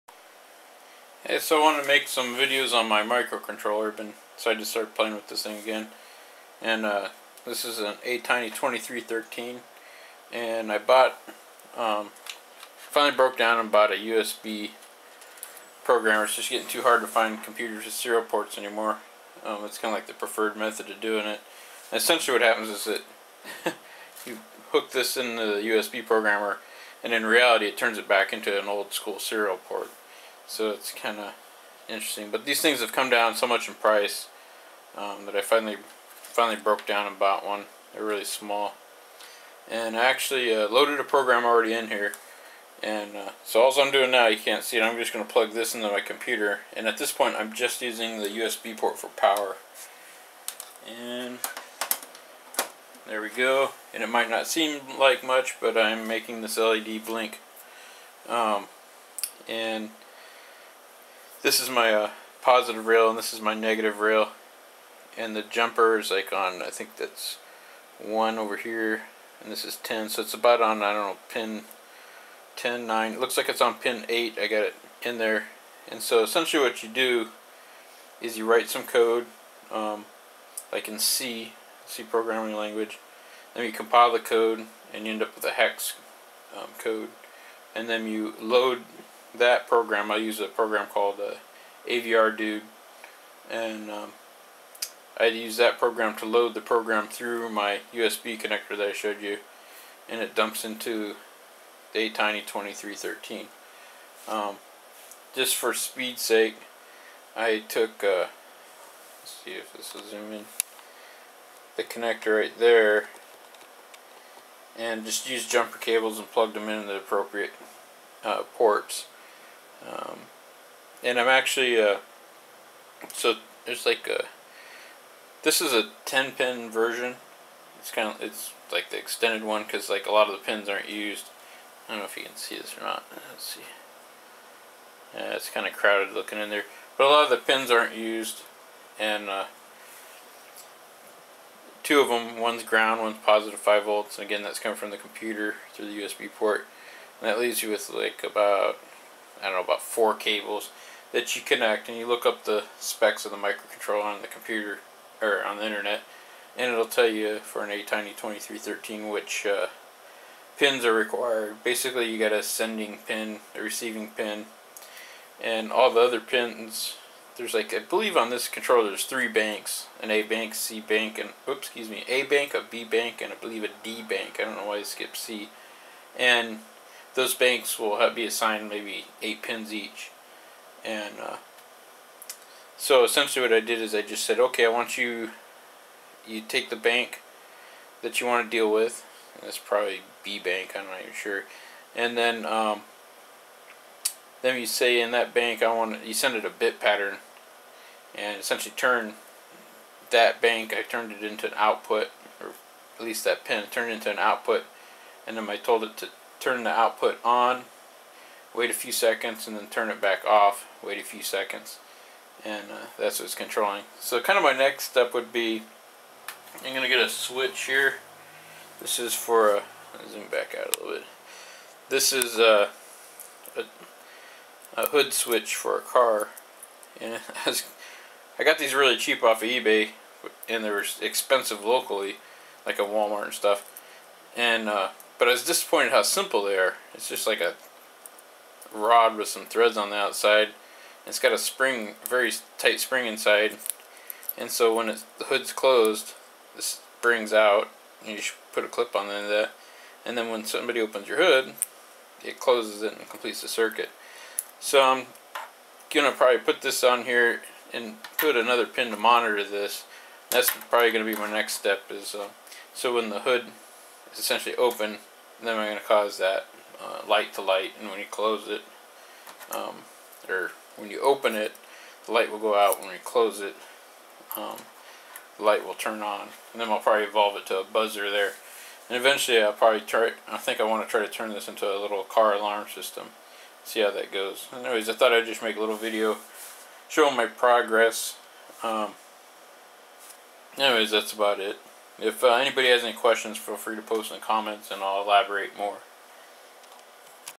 attiny2313
Programing my attiny2313 microcontroller.
Thinking of using it for a car alarm system.